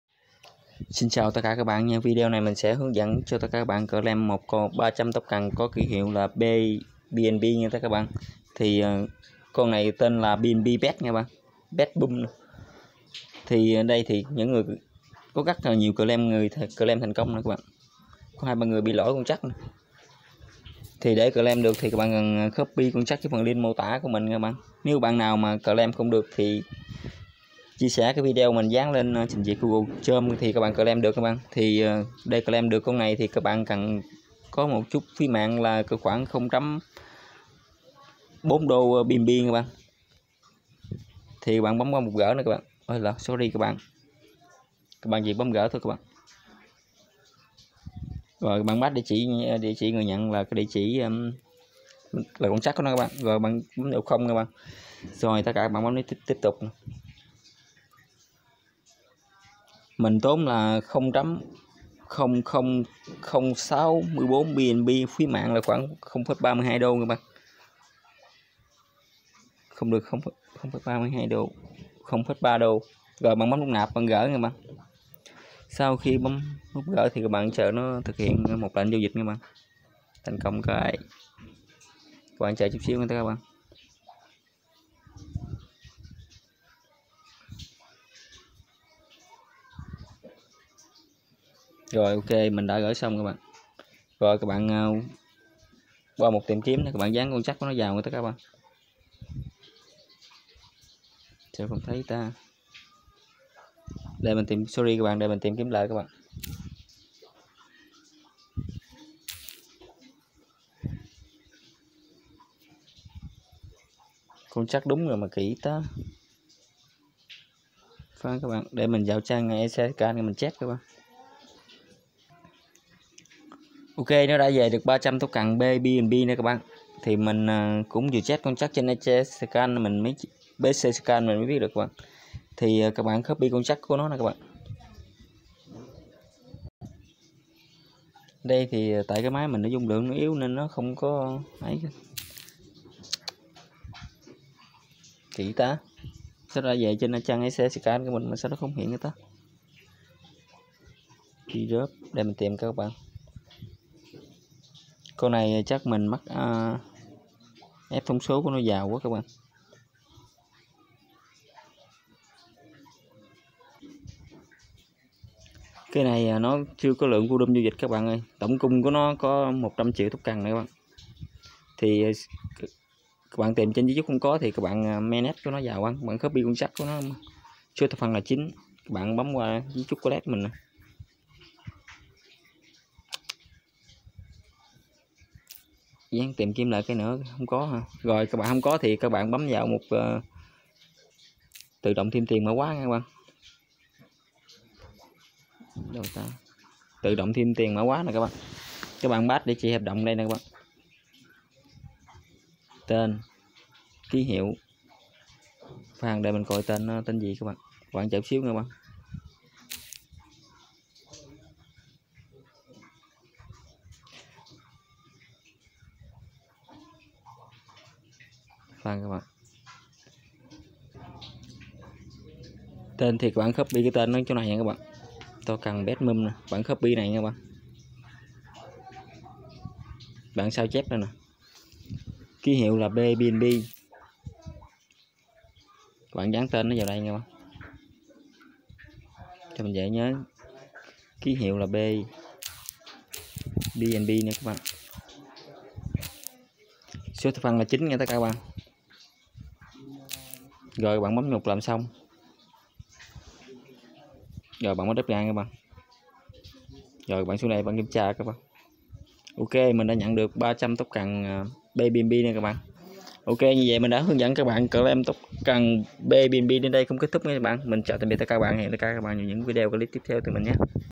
hướng dẫn Claim 300 token pBNB (mạng binace smart chain dự án tiềm năng)
con trắc PBNB

0x3514d8b004a302f35b4f5c9c81fdbb583500ea25

tên name
Token ＢＩＮＡＮＣＥ❖ＰＥＧ❖ＰＵＭ

ký hiệu pBNB

số thập phân :9
sorry ae mình nói lộn bạn nào coppy contract thì dán video mình vào trình duyệt crom coppy nhé